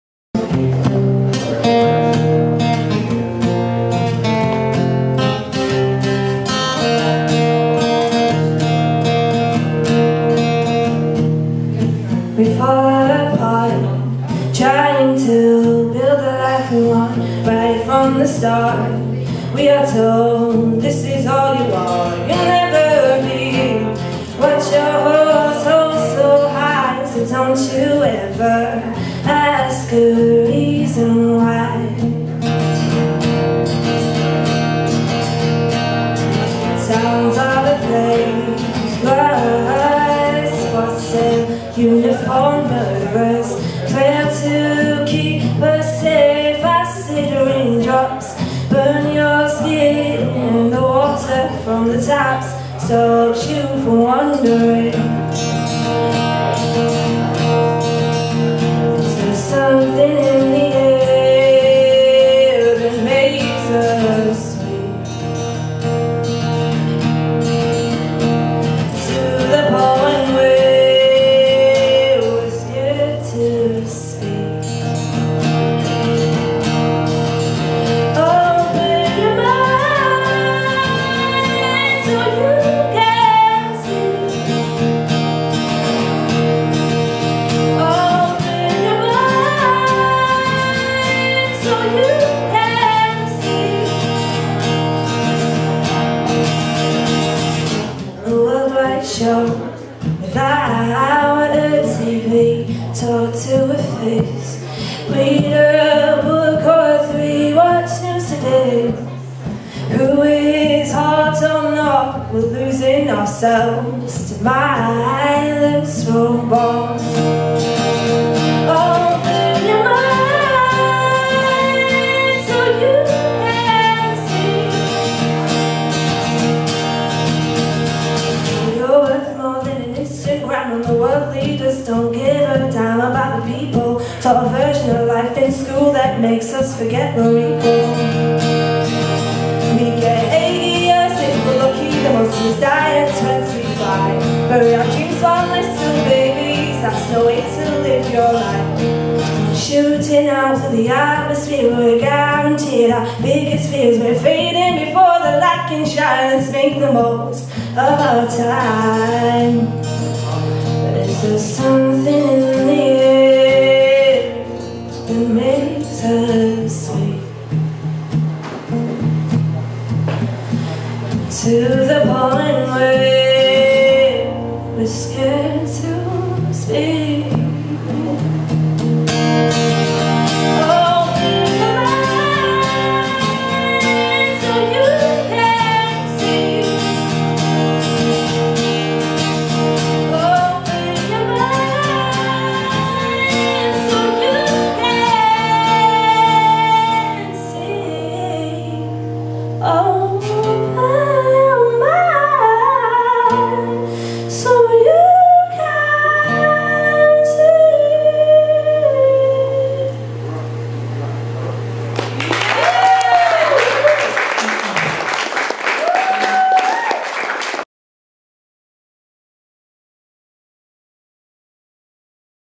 Nessa Ndukaba Yorkshire Gig Guide Featured Artist & Open Mic, Factory StreetStudio, Bradford 2016
Nessa Ndukaba Yorkshire Gig Guide Featured Artist & Open Mic, Factory Street
Studio, Bradford 2016